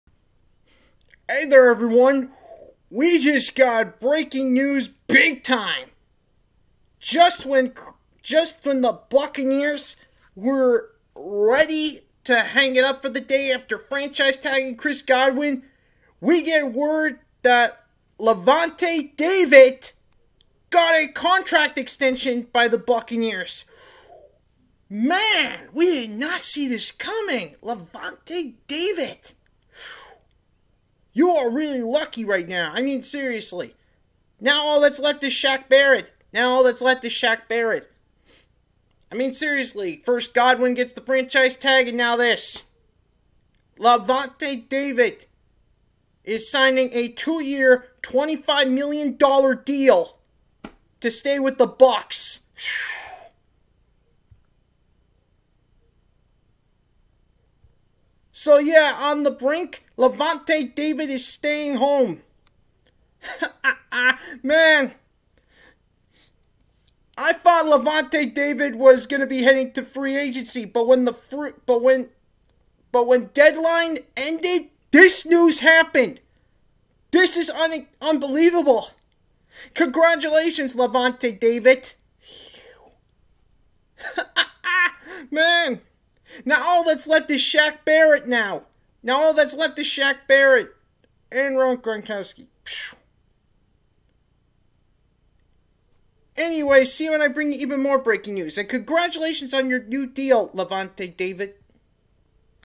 LAVONTE DAVID SECURES HIS STAY IN TAMPA BAY!!
Breaking News: Tampa Bay Buccaneers have agreed to terms with Linebacker Lavonte David on a 2-Year Contract Extension Worth $25 Million Dollars!